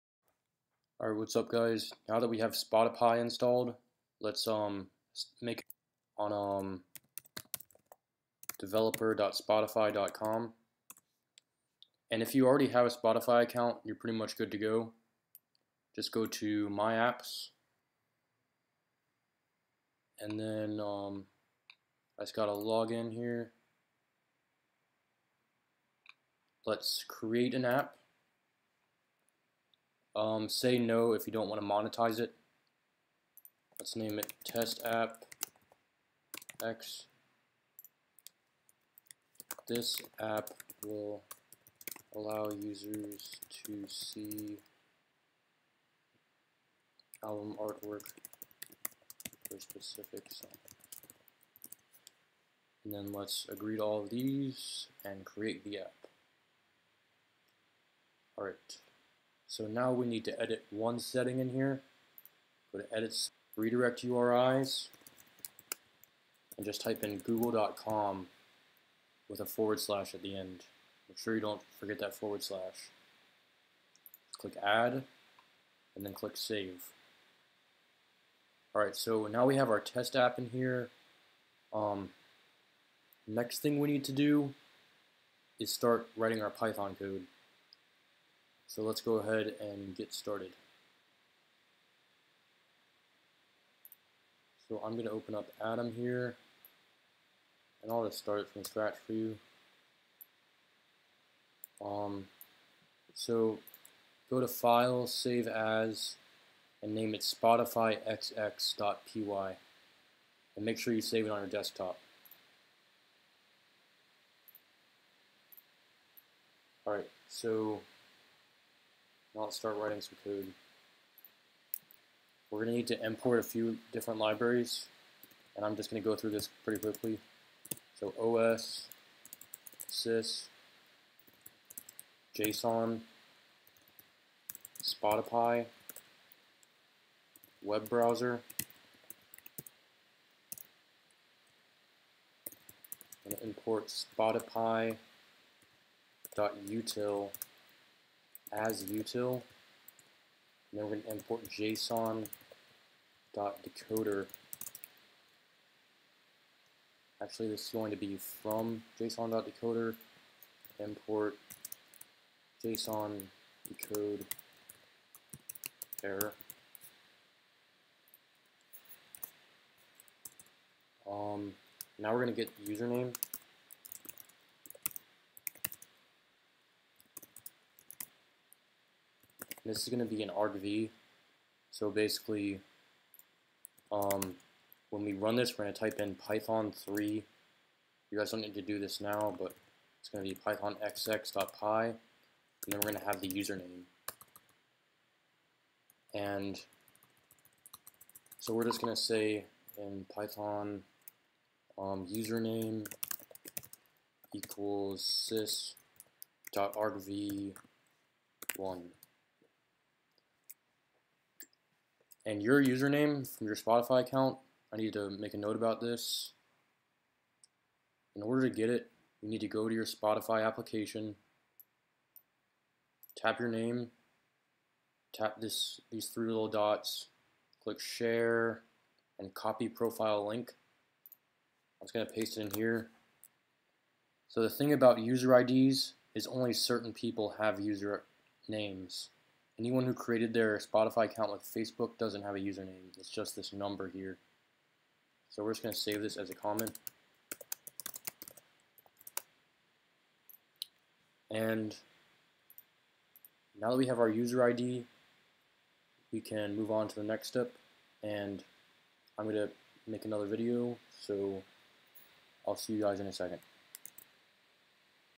2 - LEARN SPOTIPY - A Lightweight Python Spotify Library
Spotipy is a lightweight Python library for the Spotify Web API. With Spotipy you get full access to all of the music data provided by the Spotify platform.

Spotipy supports all of the features of the Spotify Web API including access to all end points, and support for user authorization.

❤ Bitcoin: 3Mm4xi2D6eJ7ReeYdwo4xYKebXERcr9RaN
❤ Litecoin: LcPi1PxCA63hGL4xkP6pfXXerbTduVDWB2